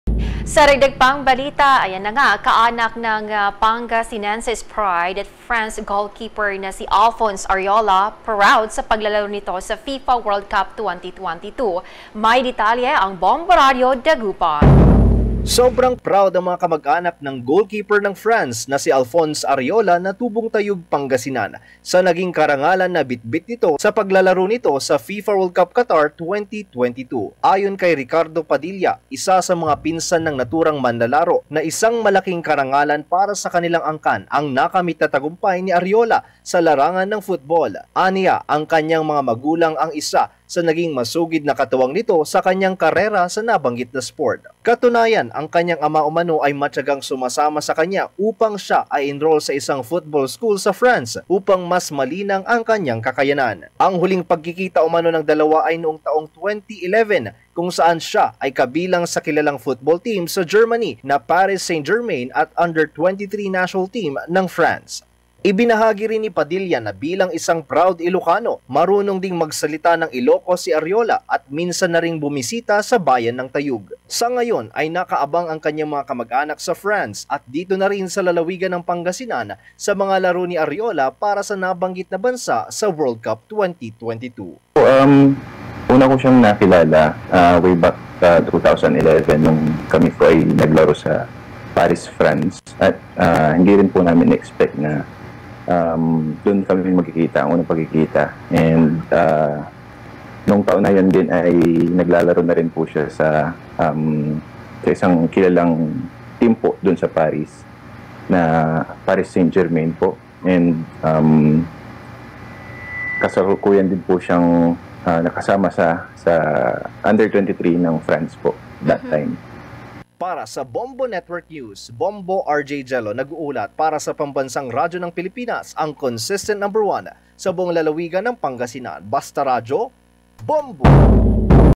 Pangasinense's pride at France goal keeper Alphonse Areola | FIFA World Cup 2022
Kaanak ng Pangasinense's pride at France goal keeper na si Alphonse Areola ‘proud’ sa paglalaro nito sa FIFA World Cup 2022